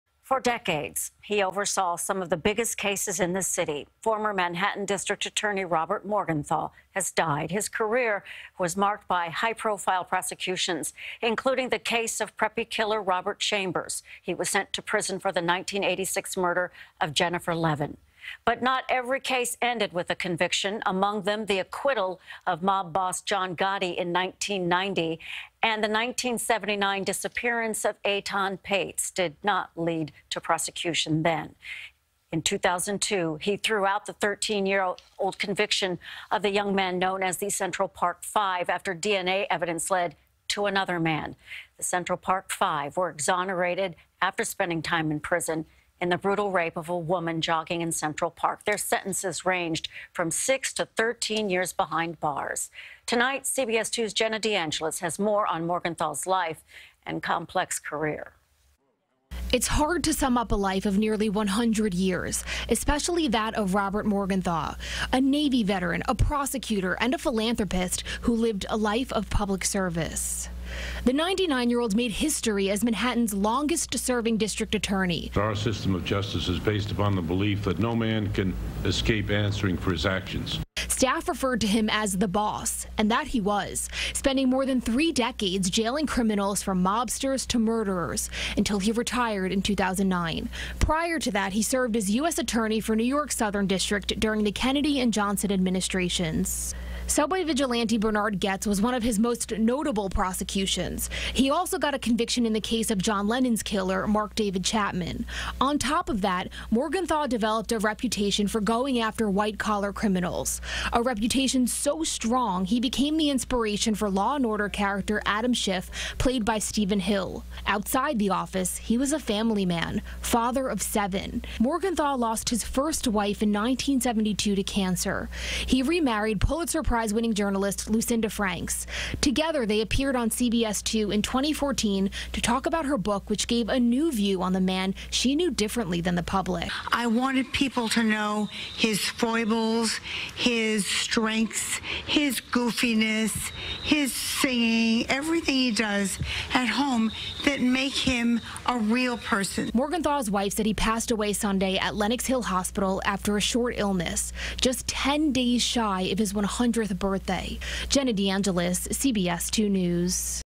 Former Manhattan DA Robert Morgenthau Dies
Former Manhattan District Attorney Robert Morgenthau has died; CBS2's Jenna DeAngelis reports.